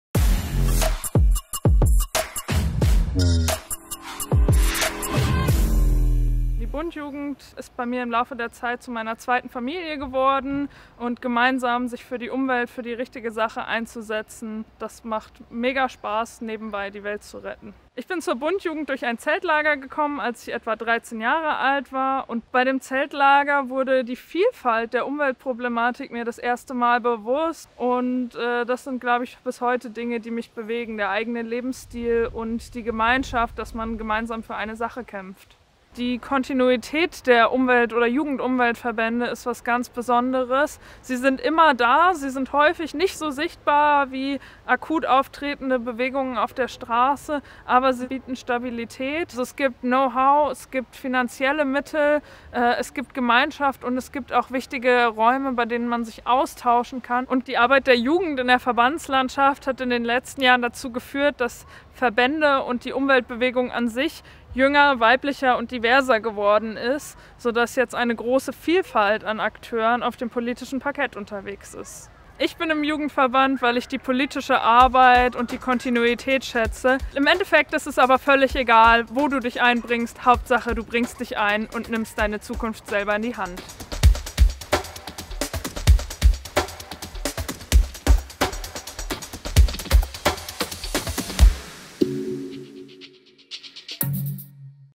Kongress "Generation Klima" - Aktiv sein im Umweltverband - Alexandra
Sich einbringen und die Zukunft selbst in die Hand nehmen - das macht Alexandra bei der BUNDjugend.
Verbandsarbeit bietet viele Möglichkeiten, da jeder von ihnen unterschiedlich aufgebaut ist und andere Schwerpunkte setzt.

Mehr Informationen zum Engagement in einer NGO oder einem Umweltverband kannst du am 26. September beim TeamUp im Rahmen des Kongresses "Generation Klima" erfahren. Hier geht's zum Programm und der Anmeldung